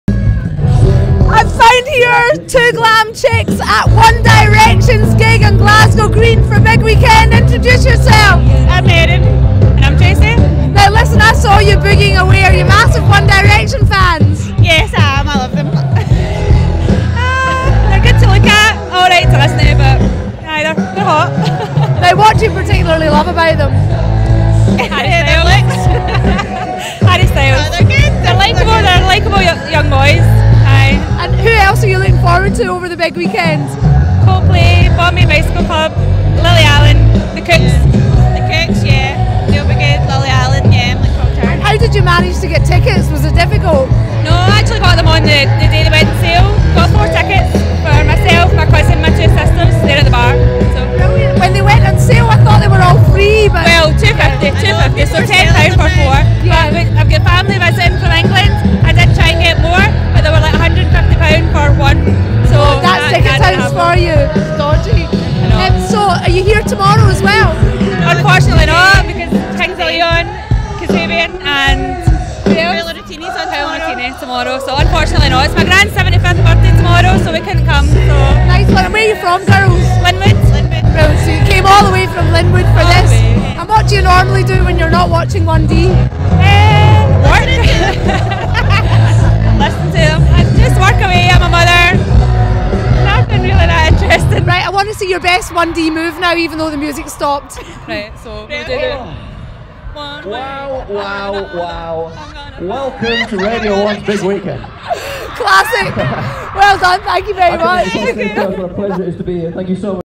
Bev chats to two girls who are loving the BBC's Big Weekend so far
Bev catches up with more fans as the BBC'S Big Weekend gets into full swing.